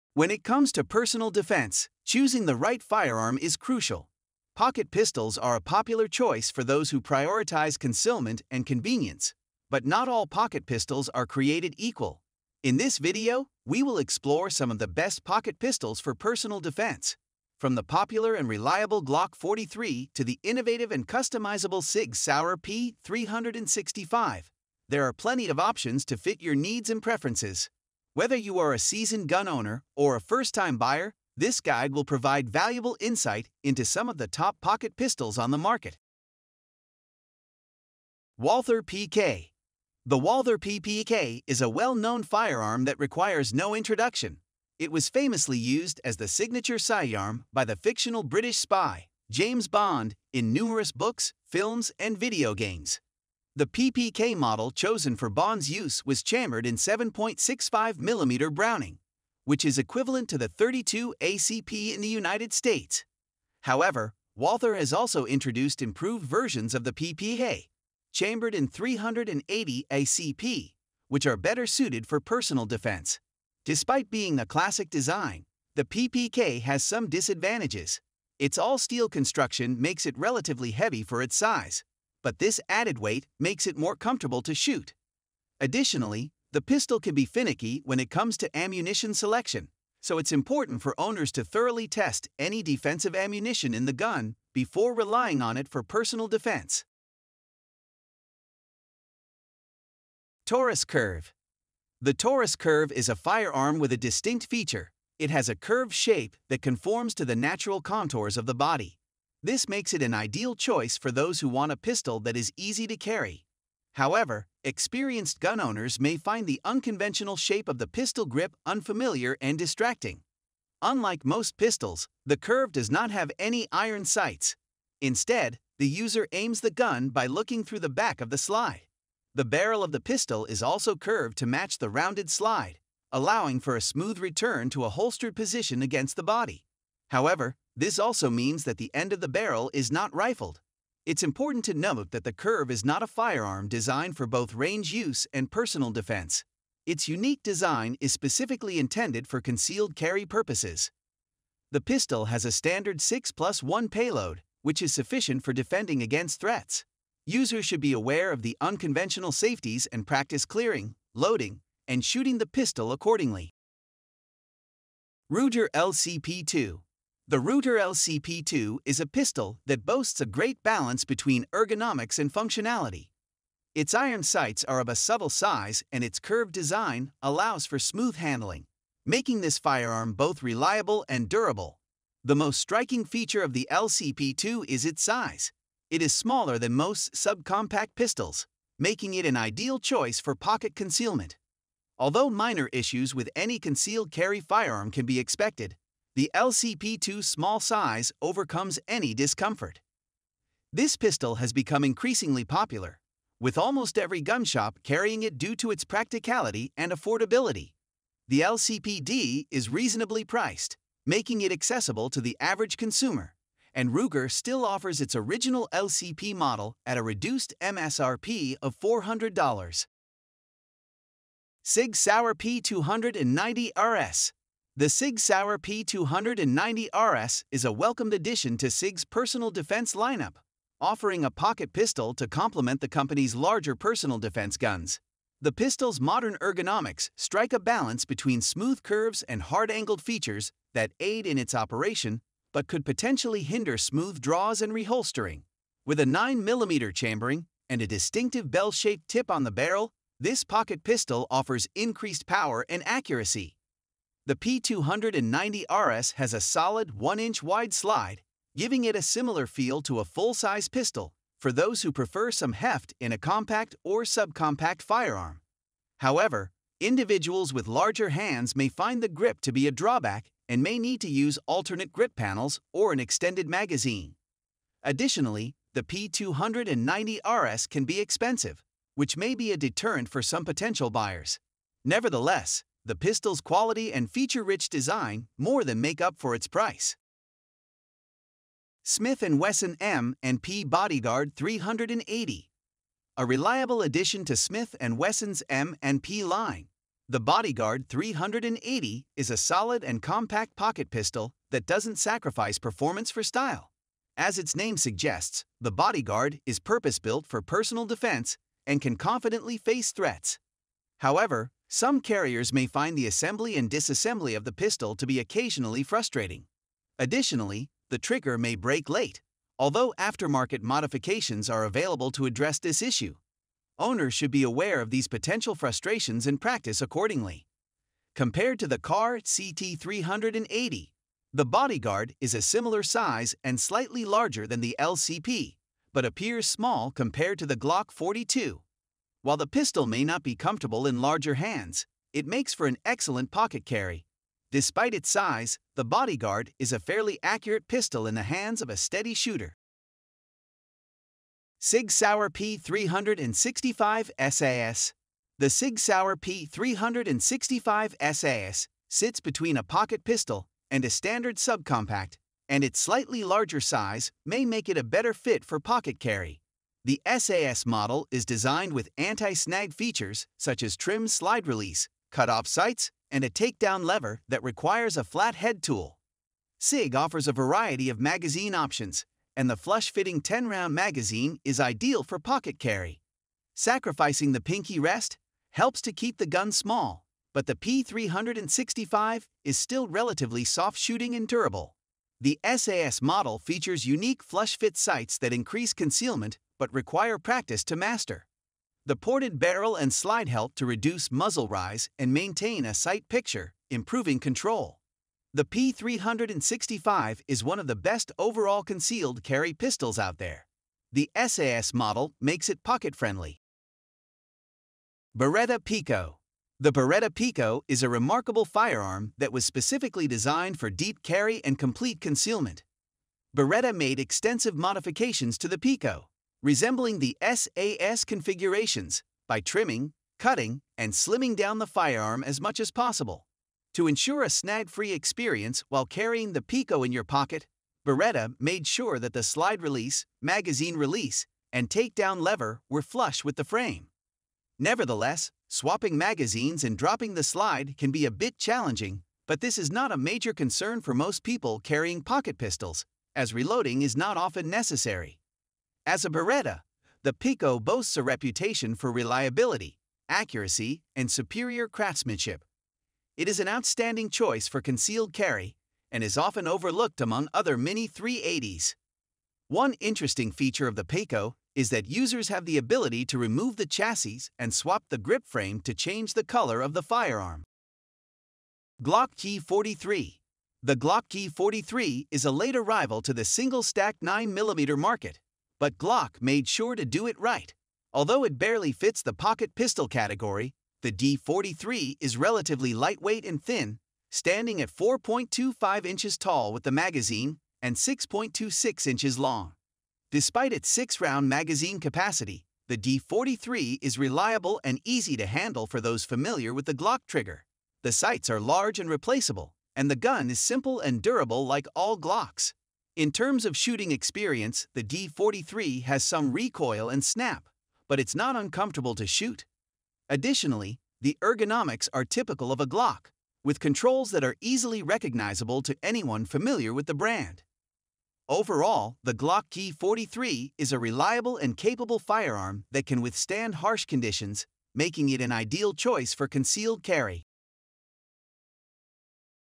TOP 8 POCKET PISTOLS FOR SELF-DEFENSE IN 2023!
When it comes to personal defense, choosing the right firearm is crucial. Pocket pistols are a popular choice for those who prioritize concealment and convenience, but not all pocket pistols are created equal. In this video, we will explore some of the best pocket pistols for personal defense. From the popular and reliable Glock 43 to the innovative and customizable sig Sauer P365, there are plenty of options to fit your needs and preferences. Whether you are a seasoned gun owner or a first-time buyer, this guide will provide valuable insight into some of the top pocket pistols on the market.